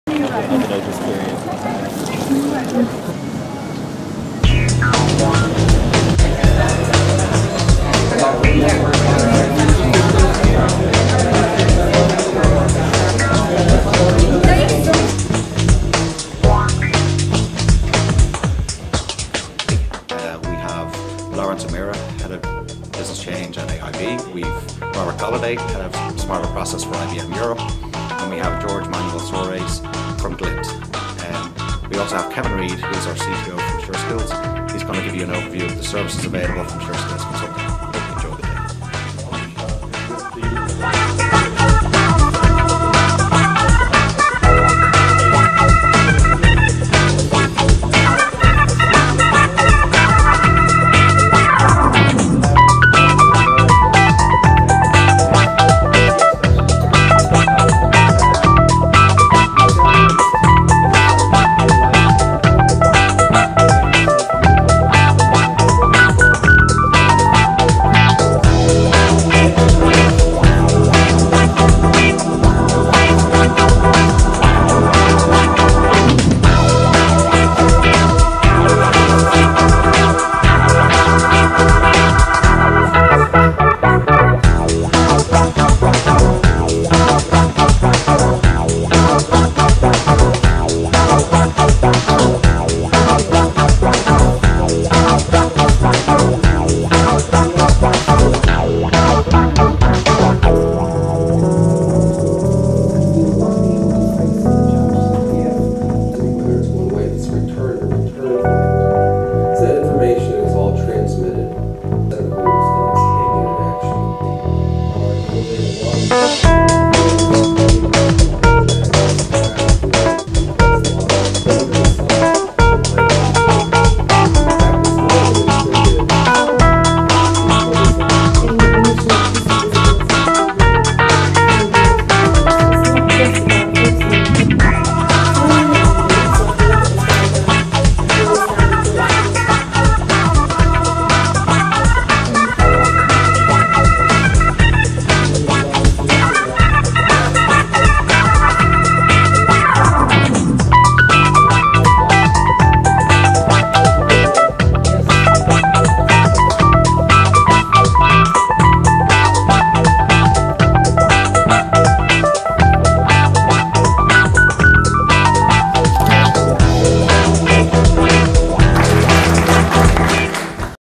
IBM Smarter Process Event
This was an invitation only event, held in the Westbury Hotel in Dublin. The focus of this event was to discuss exactly how organisations are using Smarter Process solutions to improve corporate performance.